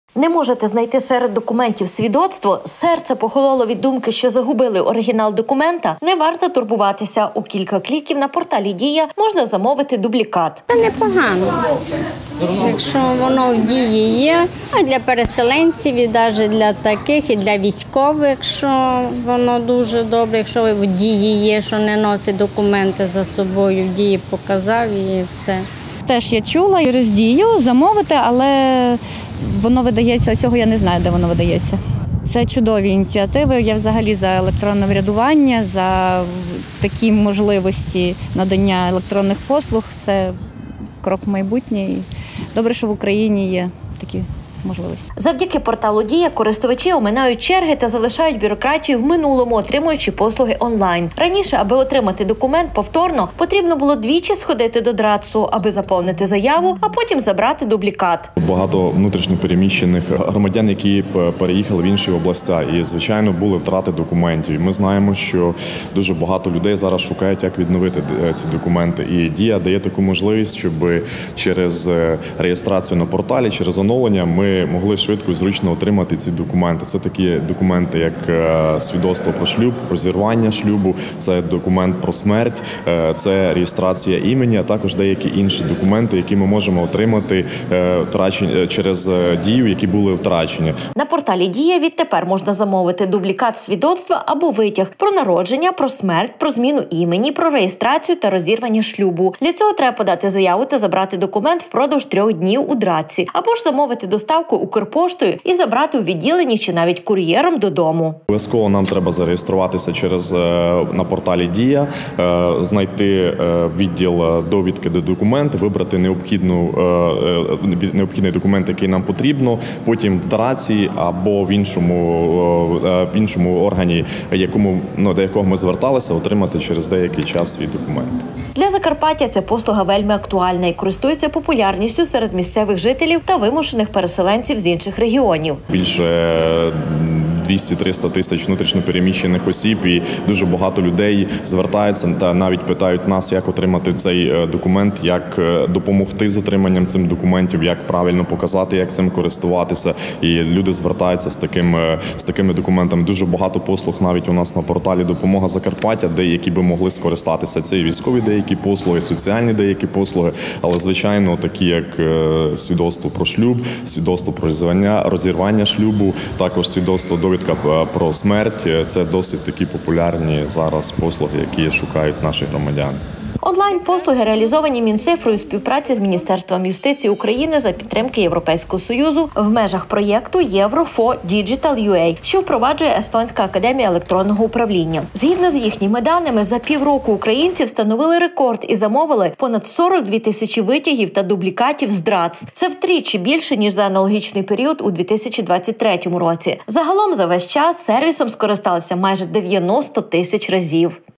Дублікати документів - у Дії
Дублікати документів можна замовити на порталі Дія. У кілька кліків, оминаючи бюрократію та черги, громадяни мають можливість подати заяву на отримання дублікатів свідоцтв та витягів. Раніше, щоб отримати документ повторно, потрібно було двічі сходити до ДРАЦСу: аби  заповнити заяву, а потім забрати дублікат. На порталі Дія відтепер можна замовити дублікат свідоцтва або витяг:  про народження,  смерть, про зміну імені, реєстрацію та розірвання шлюбу. Для цього треба подати заяву та забрати документ впродовж трьох днів у ДРАЦСі або замовити доставку Укрпоштою і забрати у відділенні чи, навіть, кур’єром додому. Для Закарпаття ця послуга вельми актуальна і користується популярністю серед місцевих жителів та вимушених переселенців з інших регіонів. Онлайн послуги реалізовані Мінцифрою в співпраці з Міністерством юстиції України за підтримки Європейського Союзу в межах проєкту EU4DigitalUA, що впроваджує естонська Академія електронного управління. Згідно з їхніми даними,  за пів року українці встановили рекорд і замовили понад 42 000 витягів та дублікатів з ДРАЦС. Це втричі більше, ніж за аналогічний період у 2023. Загалом за весь час сервісом скористались майже 90 000 разів.